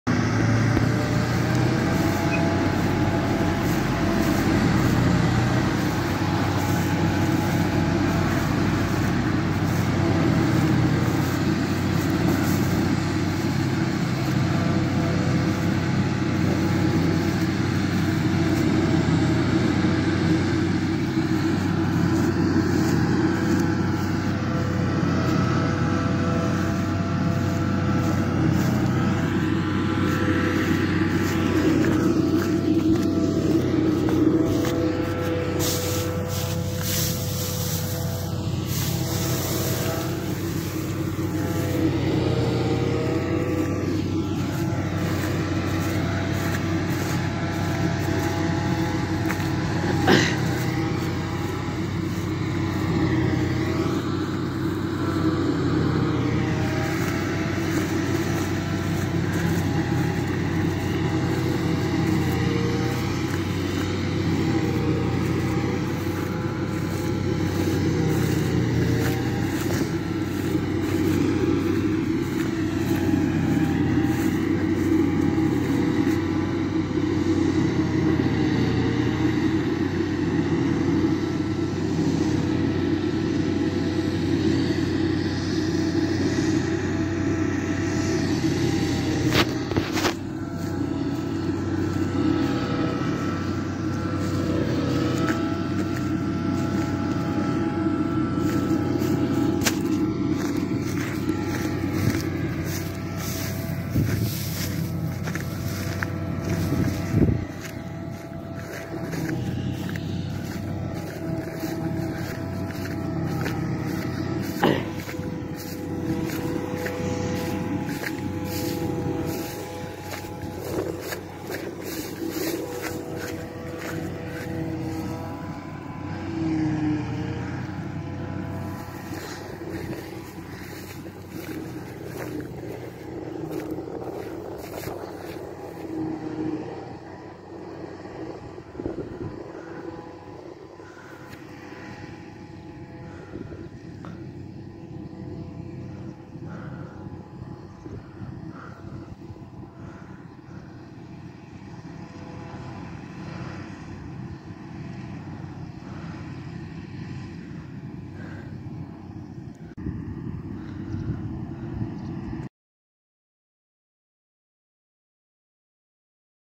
Chopping oat silage
In this video we chopping silage from oats.
We using Mengele sf 5600 chopper ,Zetor 7341 and sip Senator 26—9 loading wagon